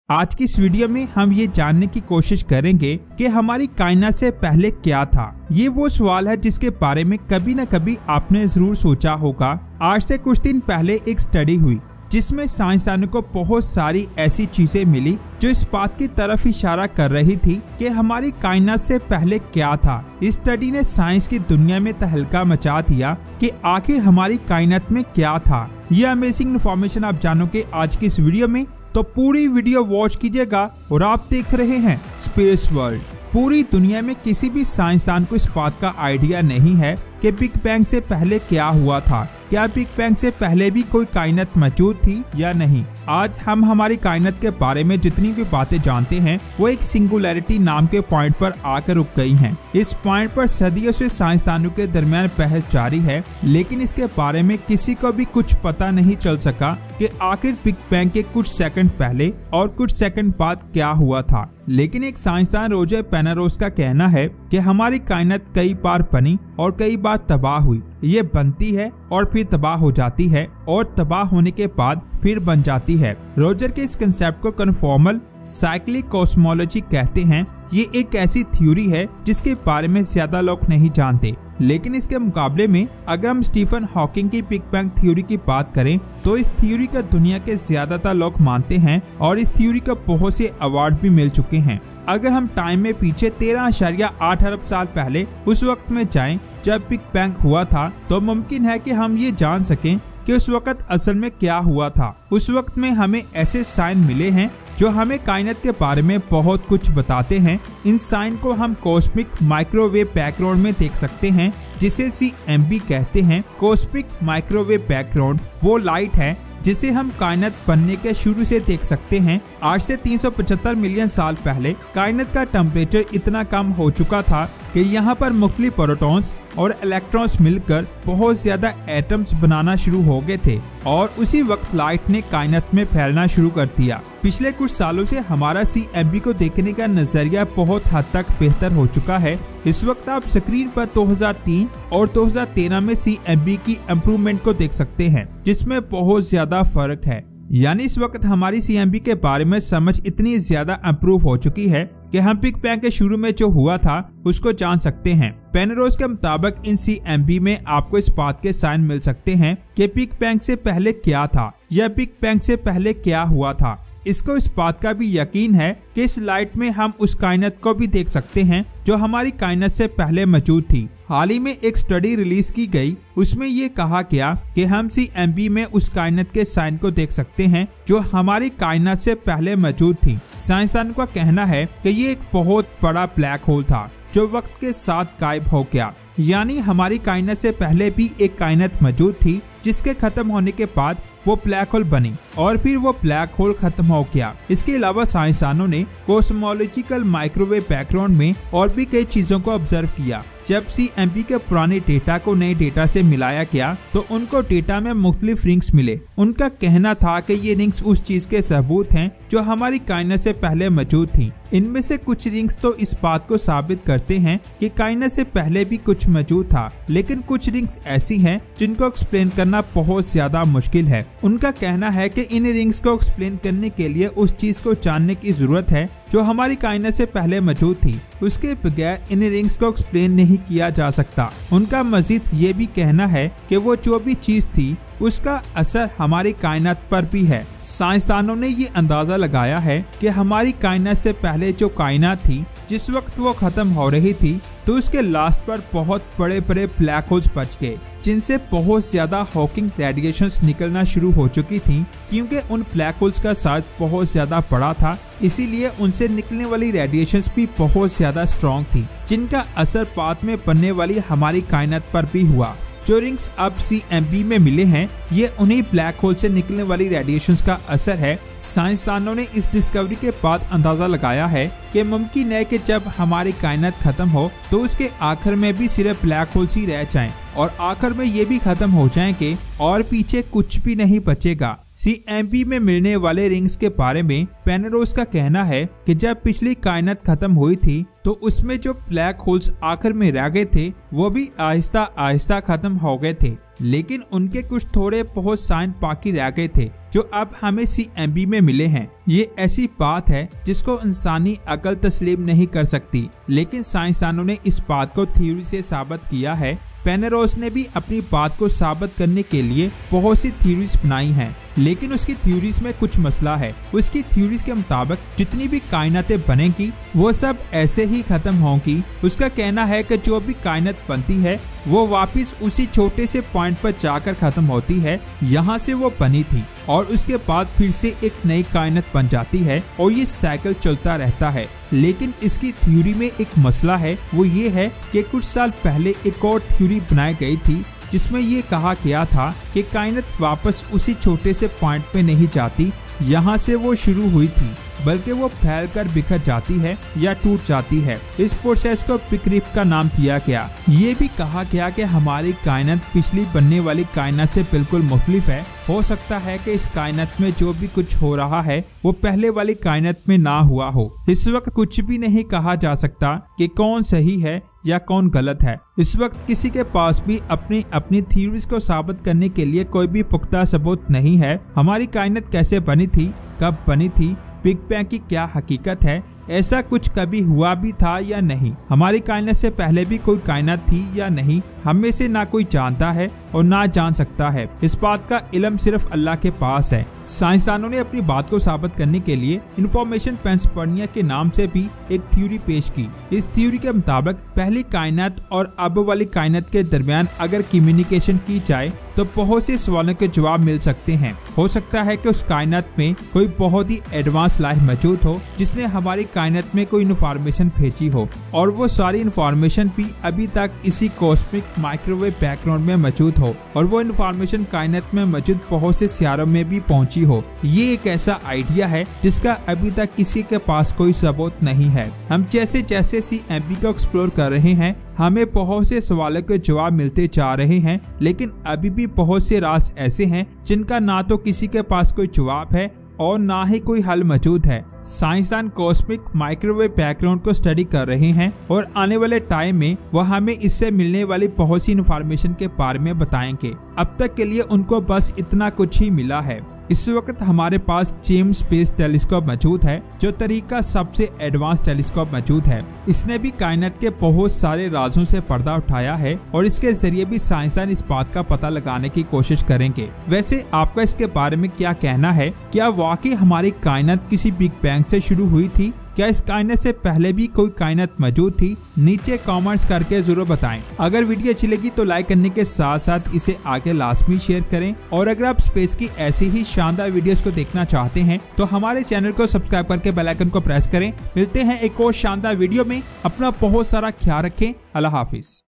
What Was Before The Universe ❙ Space World
In this Video We Will learn about the universe That What Was Before  The Universe .